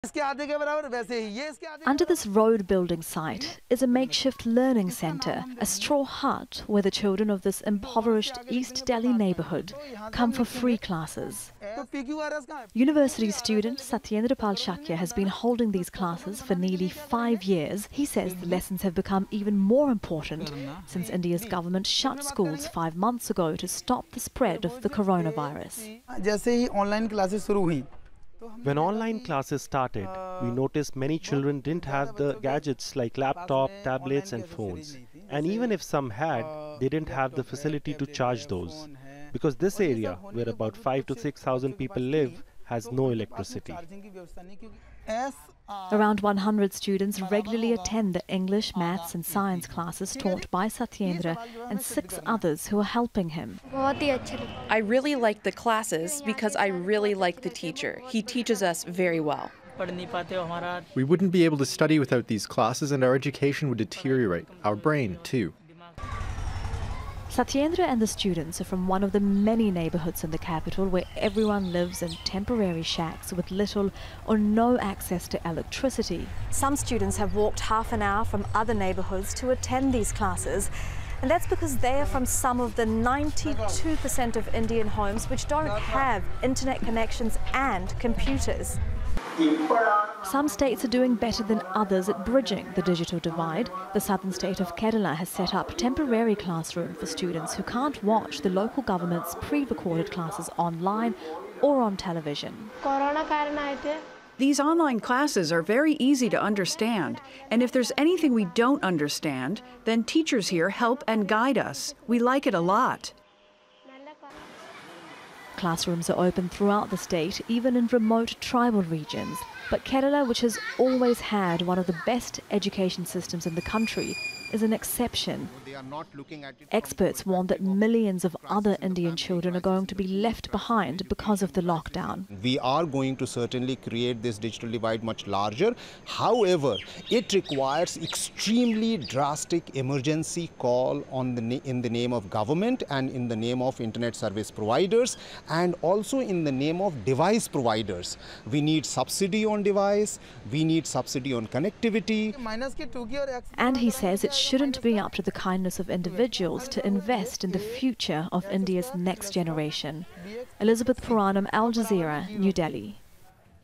Millions of children missing school in India COVID outbreak
India's schools have been shut for five months now because of the coronavirus outbreak.
Some schools have moved classes online, but millions of children do not have access to computers, phones, internet, televisions or even electricity.
Not wanting children to miss out entirely, some volunteers are setting up free classes, and the state of Kerala has set up classes that are more accessible.

Al Jazeera's Elizabeth Puranam reports from New Delhi, India.